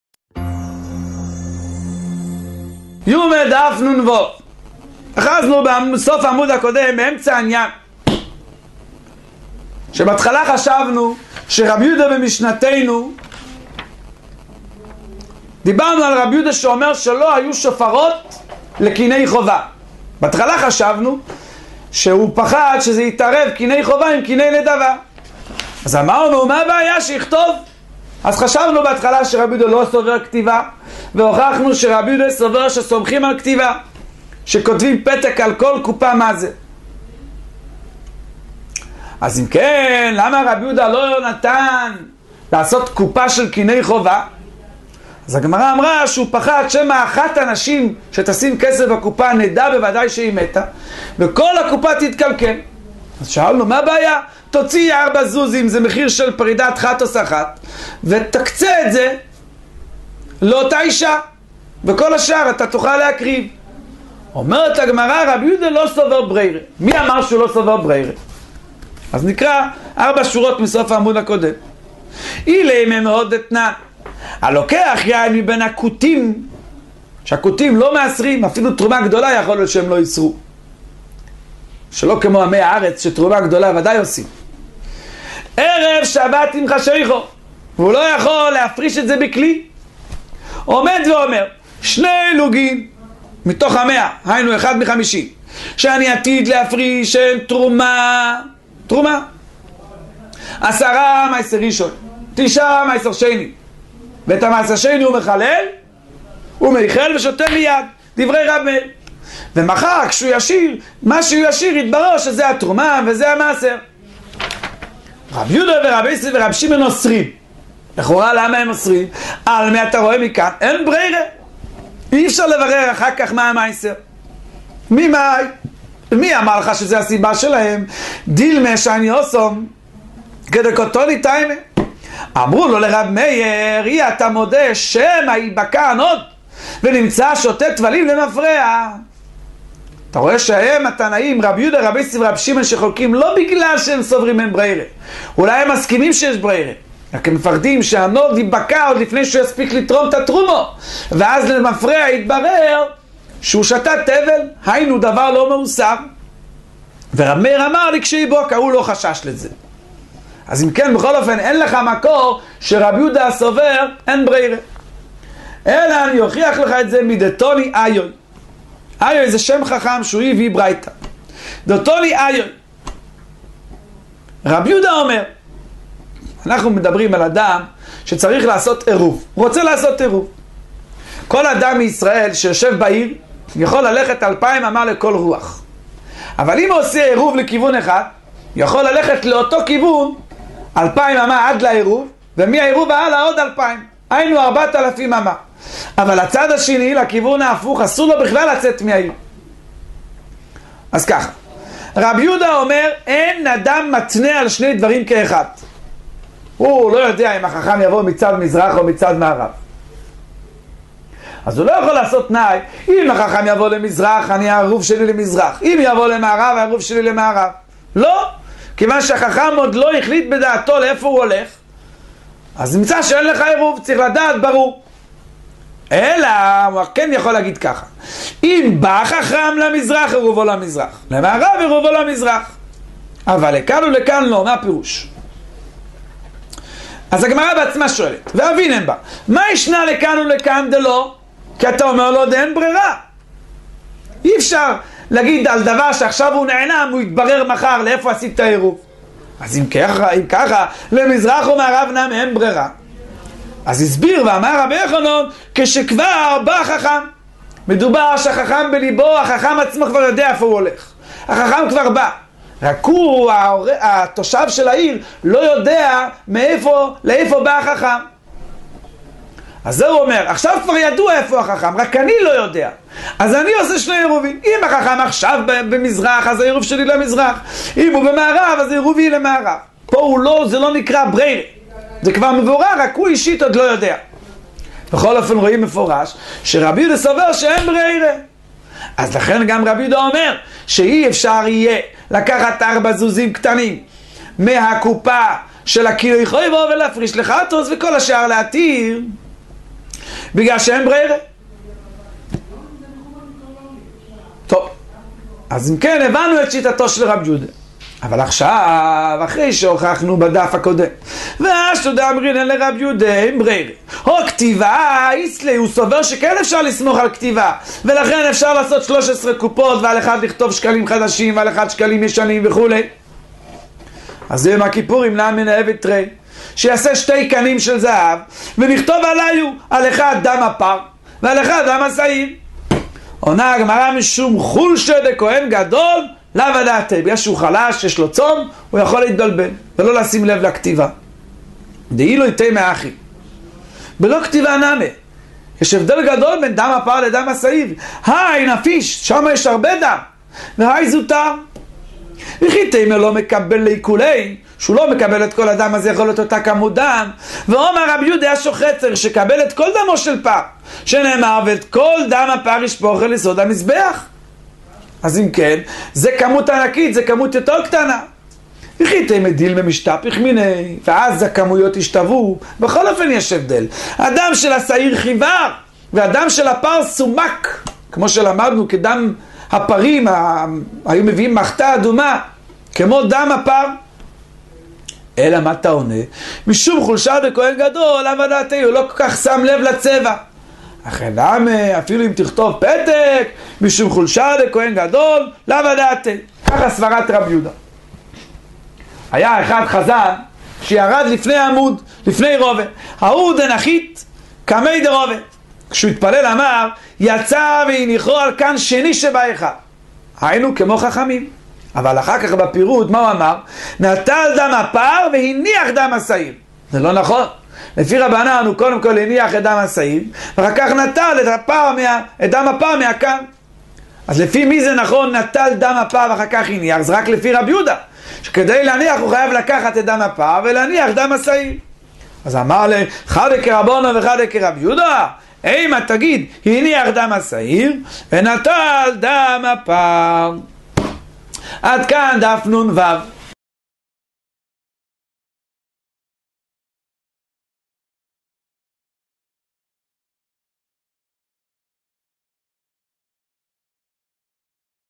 הדף היומי - מסכת יומא דף נו
שיעורי דף היומי במסכת יומא מבית "דרשו"
עם הרב אליהו אורנשטיין שליט"א
דף יומי קצר וישיבתי